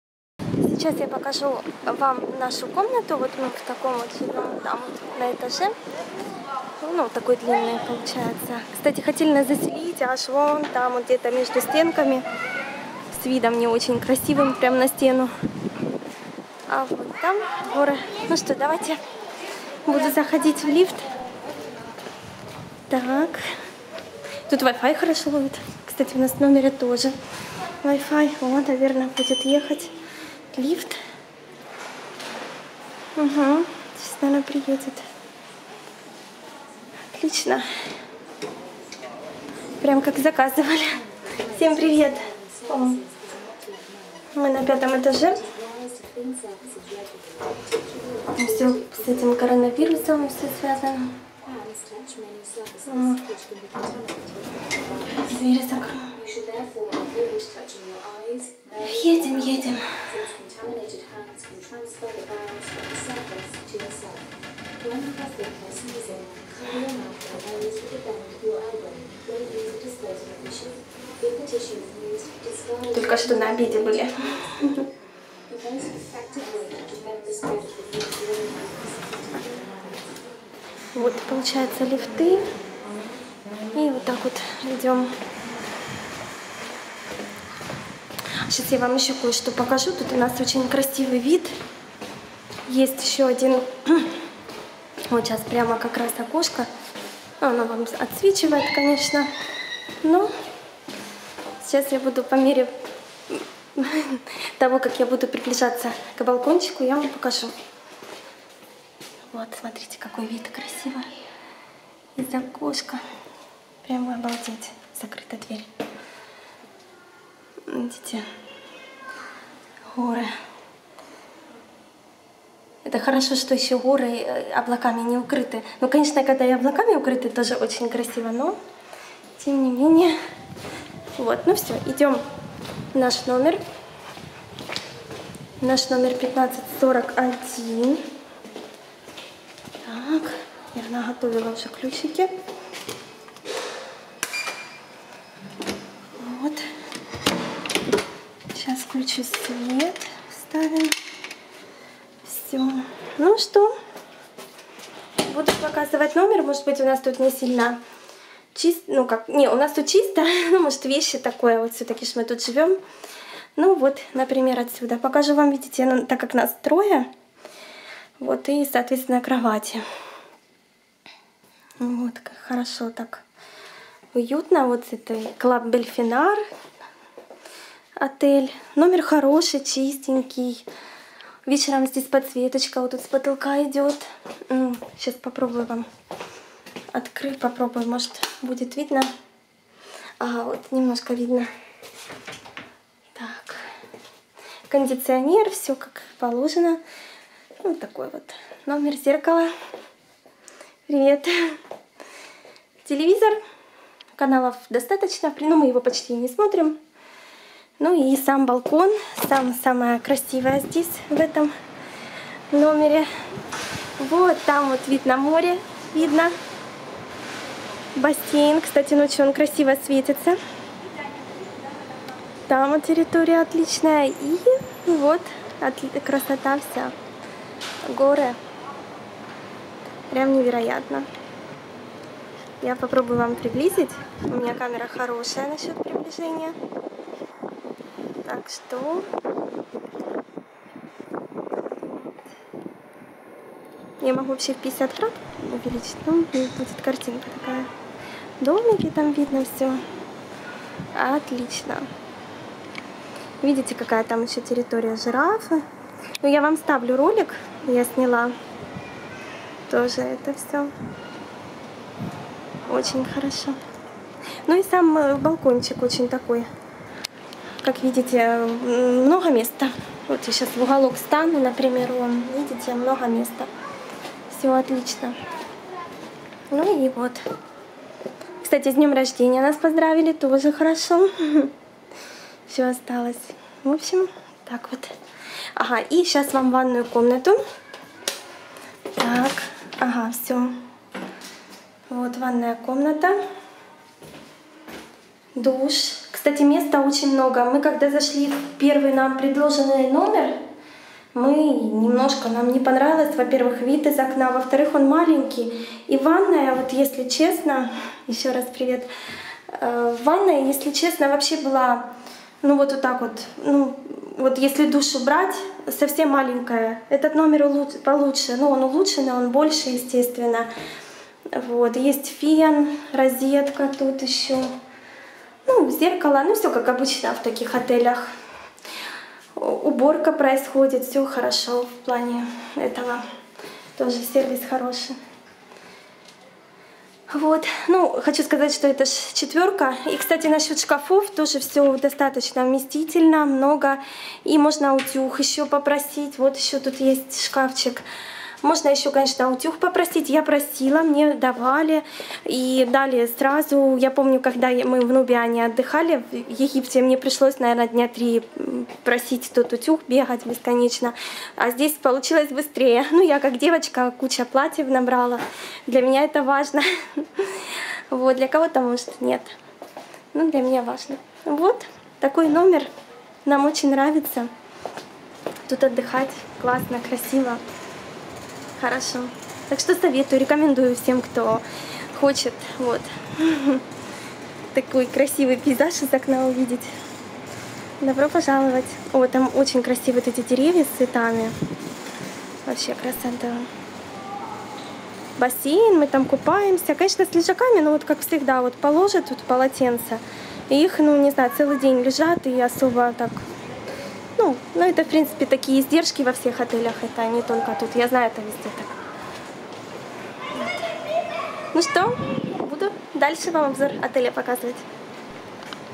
Club Hotel Belpinar 4* обзор комнаты отеля Турция 2020 (часть 2)
Наш отдых в Турции в  Hotel Club Belpinar 4* . Прогулка по отелю, наши впечатления и просто болталка